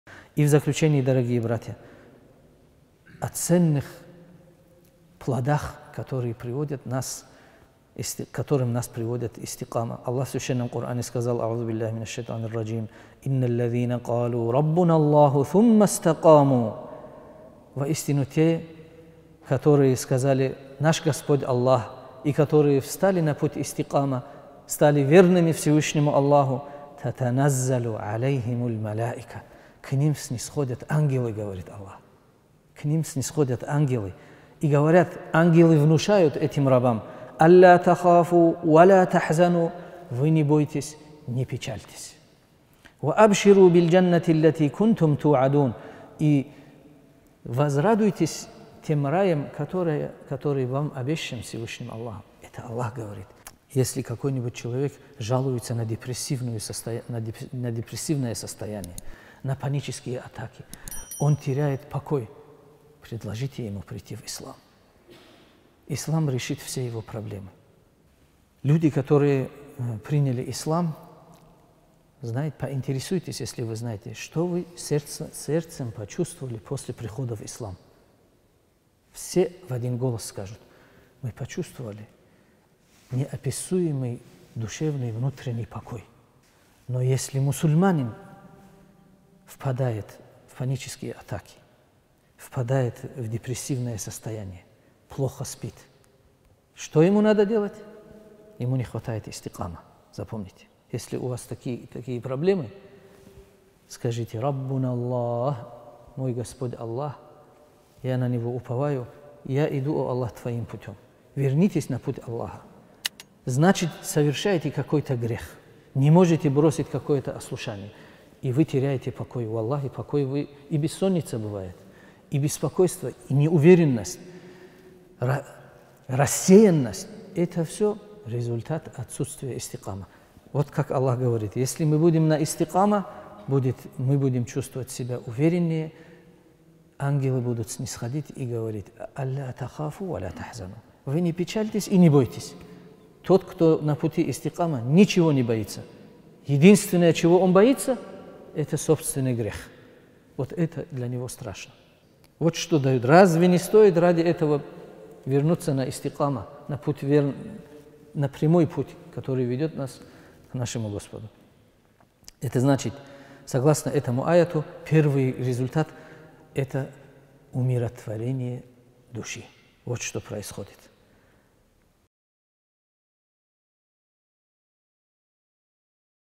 Не печалься и не бойся! / Саадуев М-Расул
Проповедь (отрывок) после утреннего намаза в центральной мечети г. Каспийск